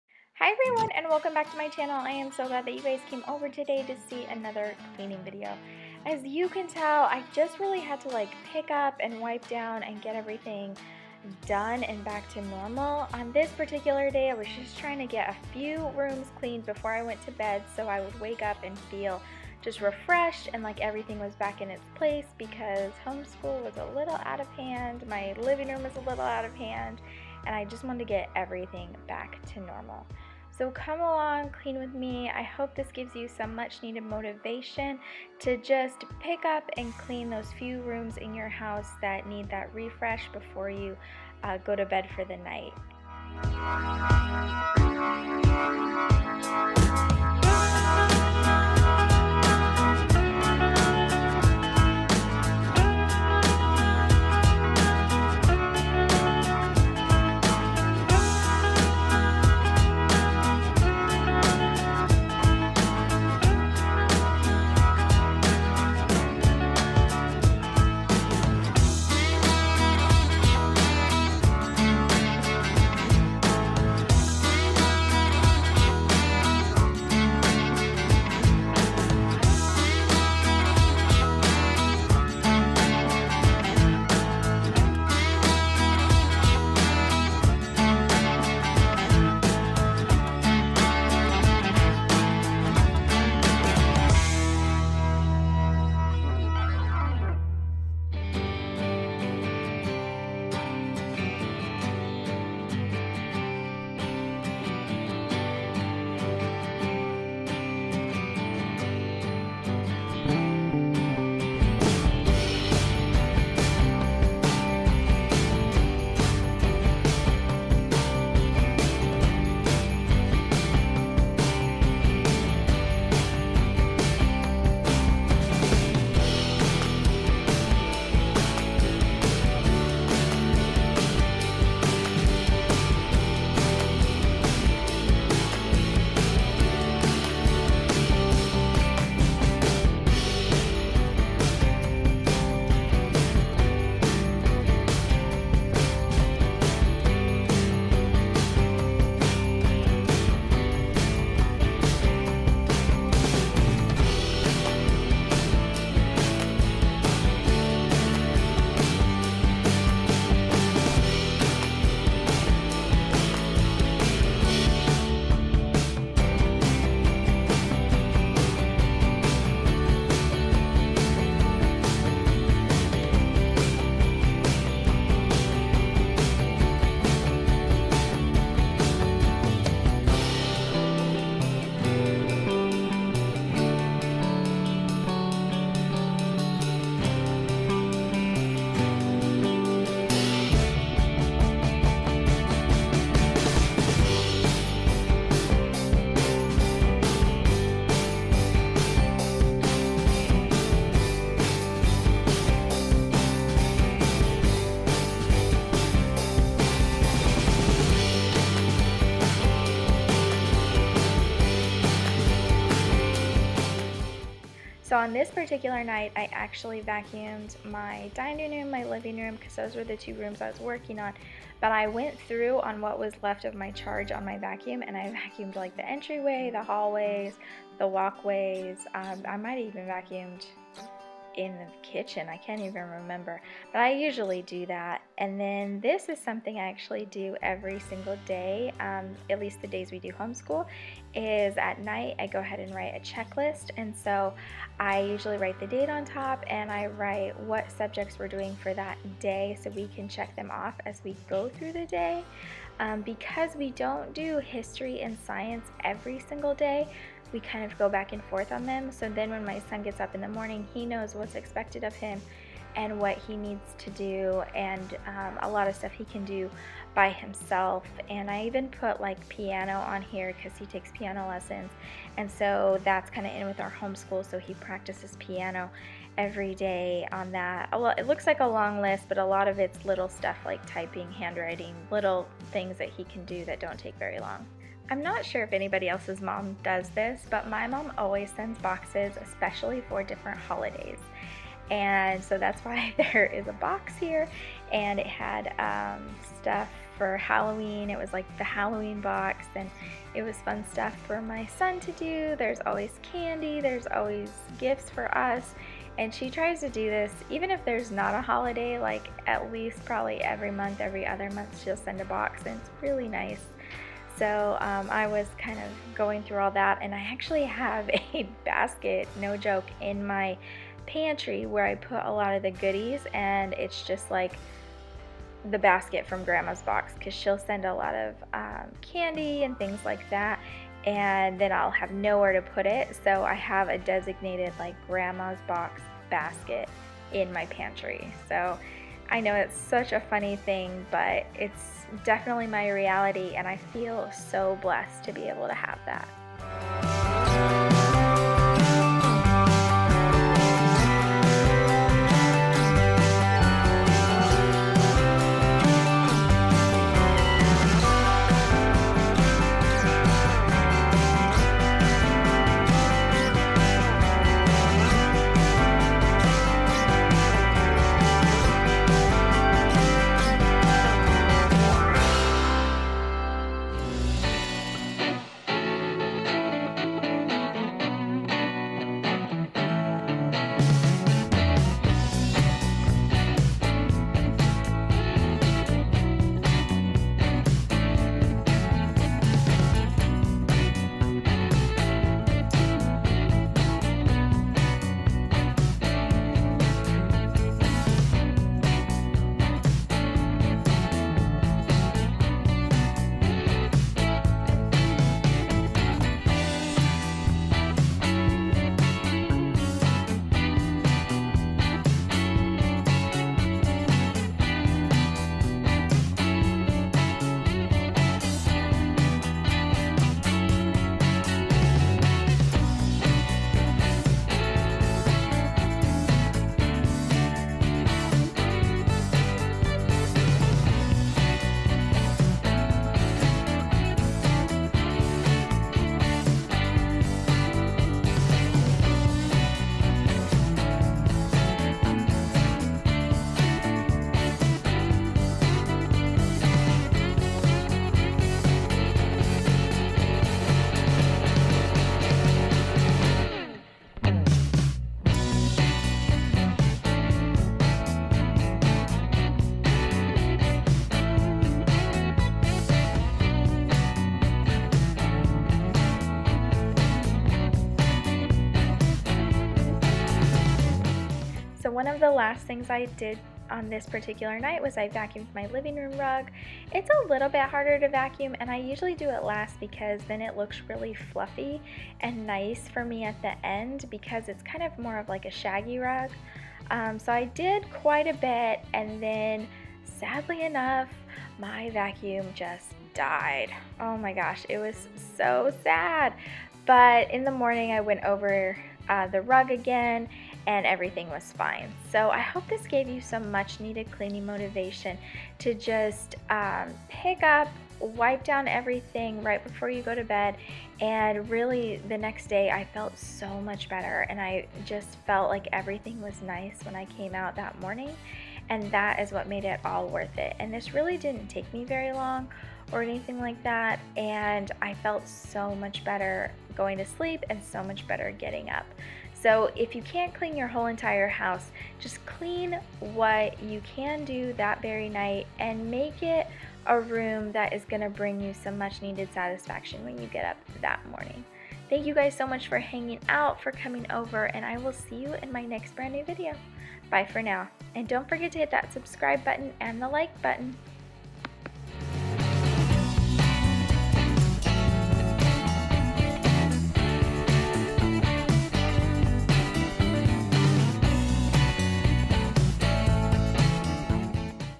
POWER HOUR CLEAN WITH ME! | EVENING CLEANING MOTIVATION | SPEED CLEAN WITH ME | SAHM
Hope you enjoy this Power Hour Clean with Me! This is power hour definitely helped get my messy house by to normal while hopefully you all cleaned with me. I hope this speed clean with me gives you some much needed cleaning motivation. Sometimes power hour cleaning is just the ticket on those busy days in life. See you all in my next Speed clean with me video!